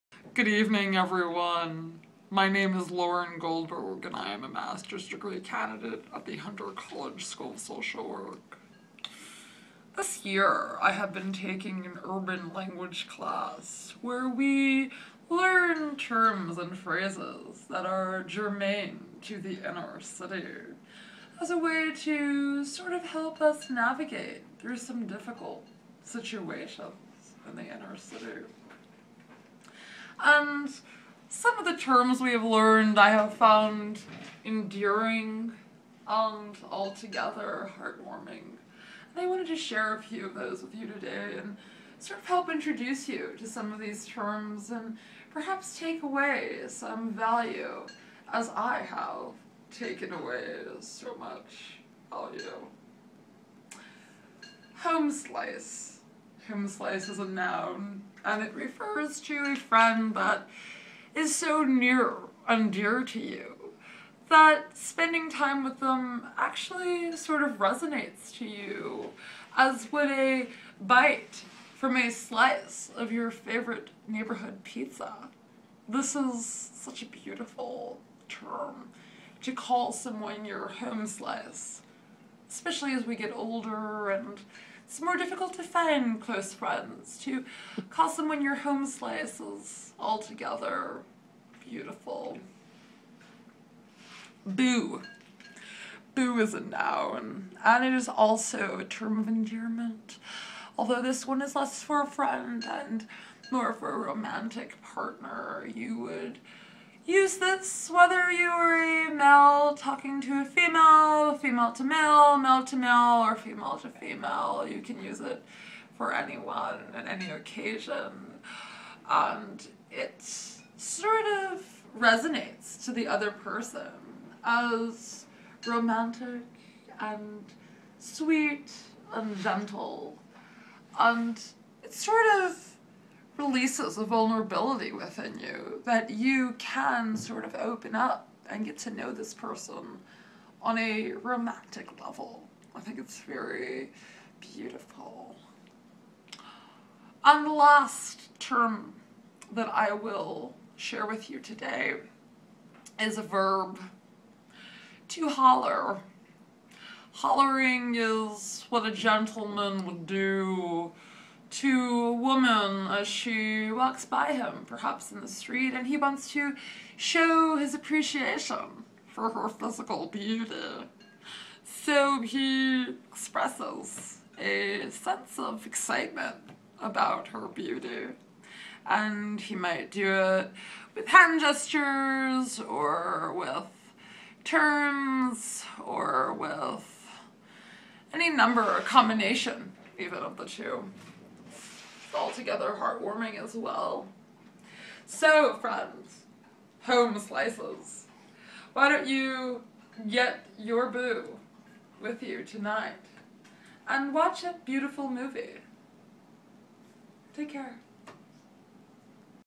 Urban Terminology with Lauren Goldberg
Lauren is particularly endeared by 3 new terms she learned in her Urban Language class: homeslice, boo, and holler. Here, she provides their parts of speech and definitions.